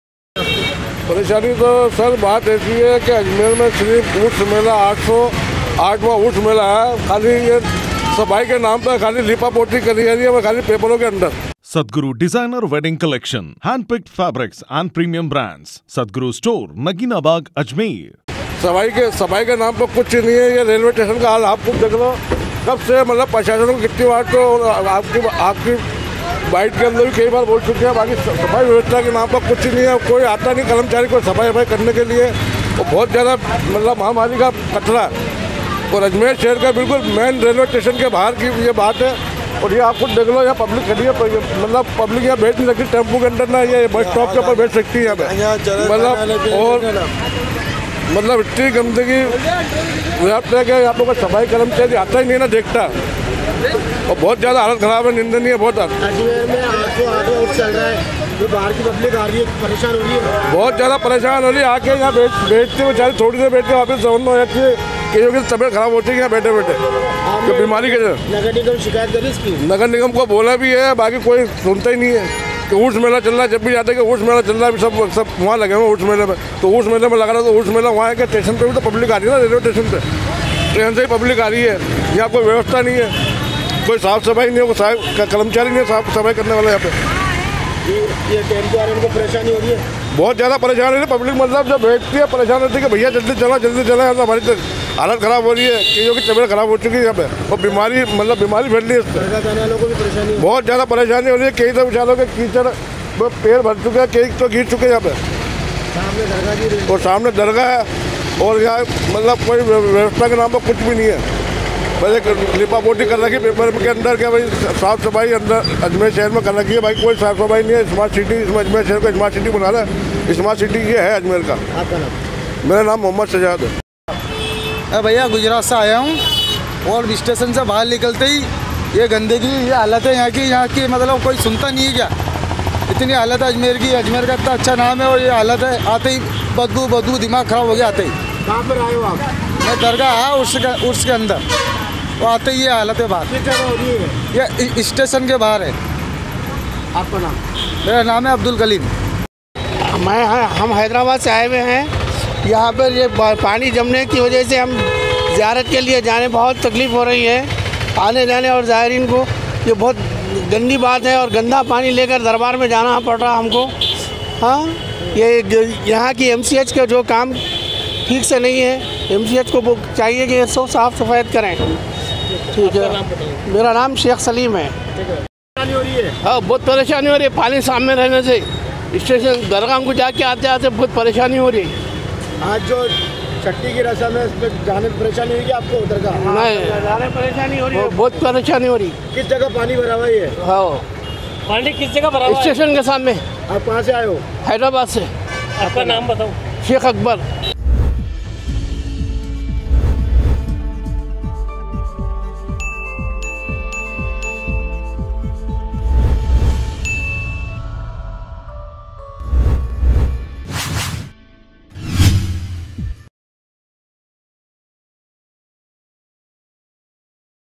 AJMER NEWS | गंदगी और अव्यवस्था से रेलवे स्टेशन की रोड हुई बेहाल | MTTV INDIA
गंदगी और अव्यवस्था से रेलवे स्टेशन की रोड हुई बेहाल
स्टेशन के मुख्य द्वार पर लोग कर रहे है खुले में शोच
गंदगी के आलम से जायरीन और शहरवासी हो रहे परेशान
नगर निगम और प्रशासन नहीं ले रहा कोई सुध